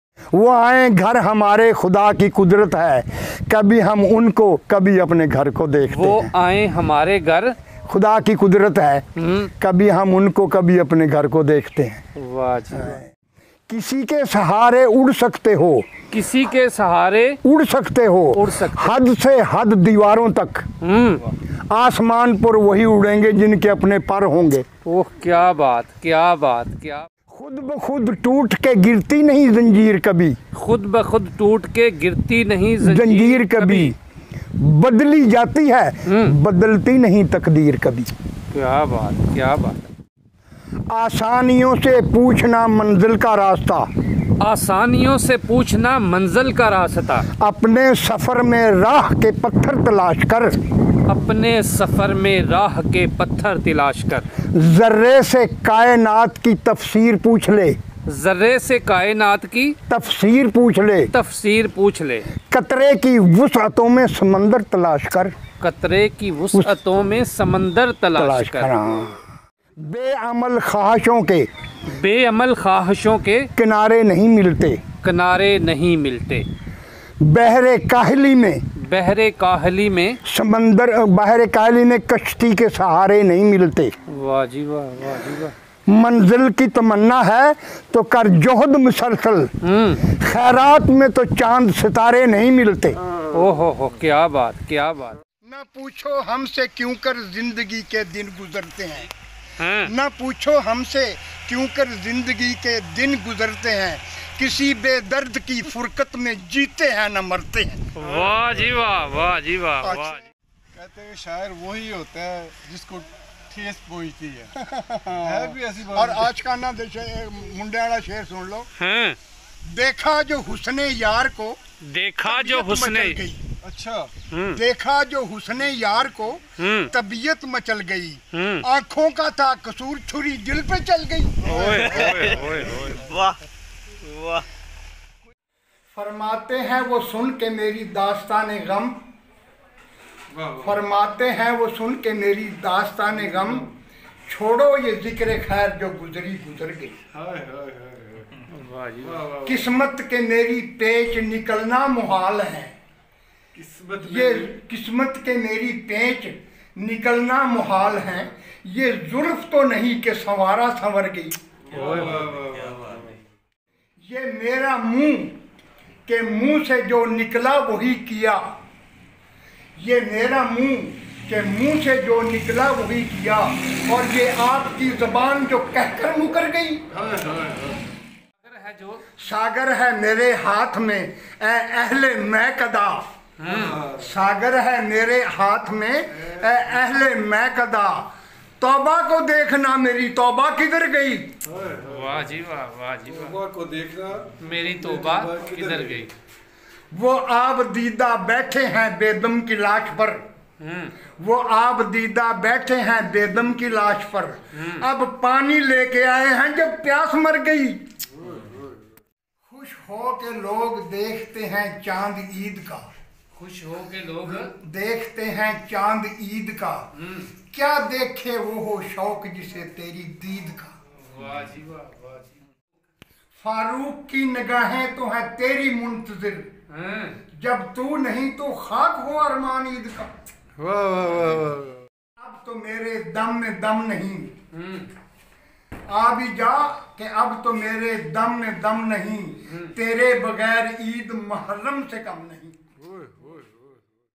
Kamal Kar Diya || 90 Year Old Poet Man
90 Year Old Poet Man Rana Muhammad Yaseen Khan
نوے سالہ بزرگ نے شعر پڑھتے ہوئے کمال کر دی